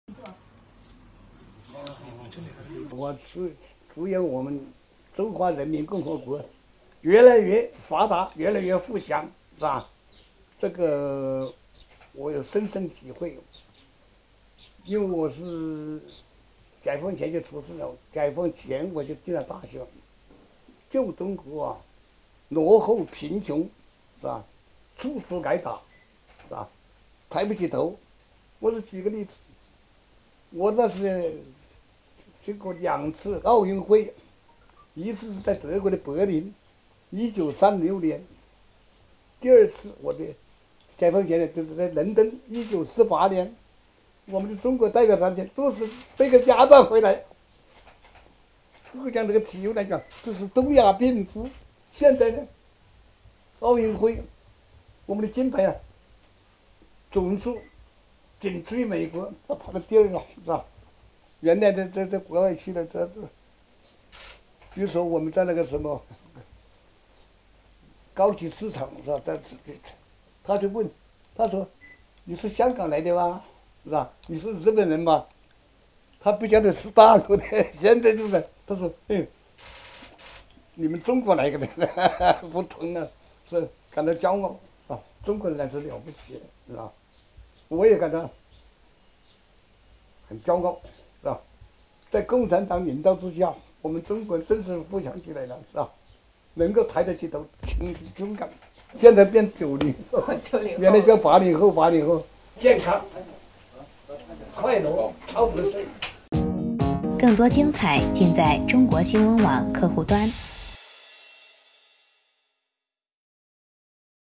袁隆平：我是“90后” 现在要健康快乐超百岁
9月5日，湖南长沙。“杂交水稻之父”袁隆平在接受记者集体采访时，饱含深情地祝愿祖国越来越富强。他说，自己出生在旧社会，对新旧中国的变化深有体会，如今中国的发展让他感到很骄傲。当在场媒体祝福“90后”的他健康长寿时，袁隆平笑称现在要健康快乐超百岁。